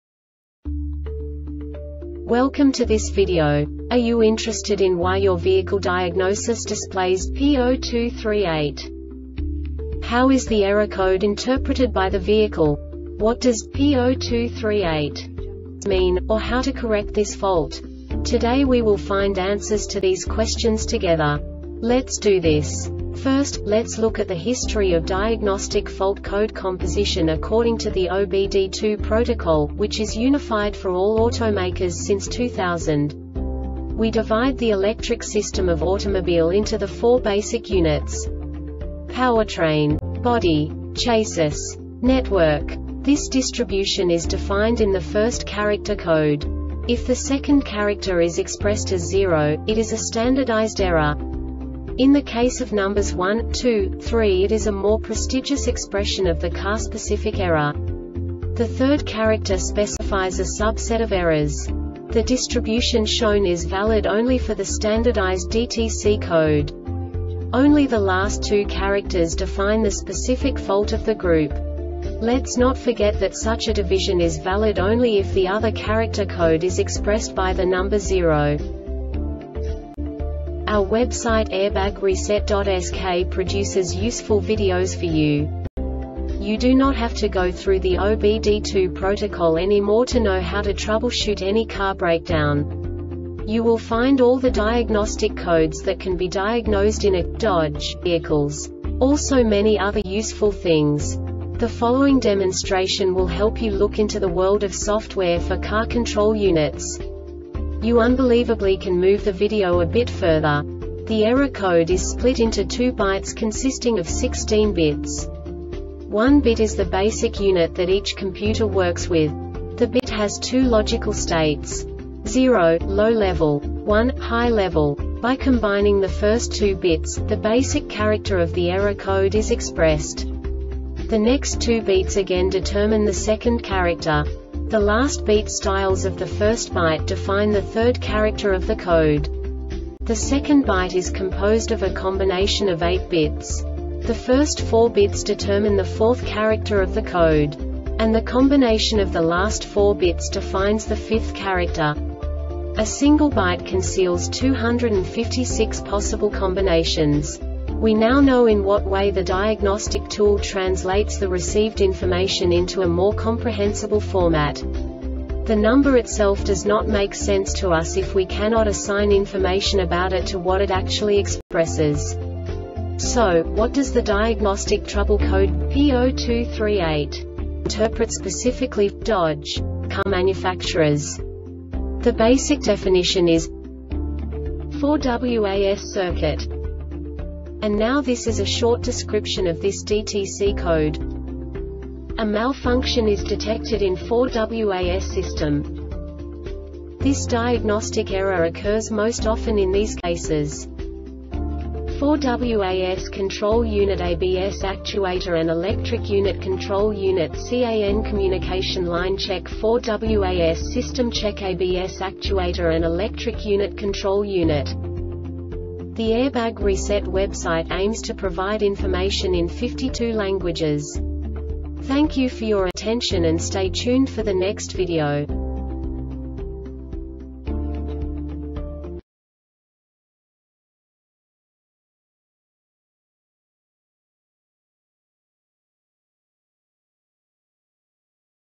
DTC Dodge P0238 Short Explanation
Contents:
0:21 Basic DTC analysis according to OBD2 protocol standard.
1:48 Insight into programming
2:58 A diagnostically understandable description of the Diagnostic Trouble Code
3:05 Basic error definition

Make
Dodge
Code
P0238
Definition
4WAS Circuit
Description
A malfunction is detected in 4WAS system
Cause
4WAS control unitABS actuator and electric unit control unitCAN communication line
Diagnosis
Check 4WAS systemCheck ABS actuator and electric unit control unit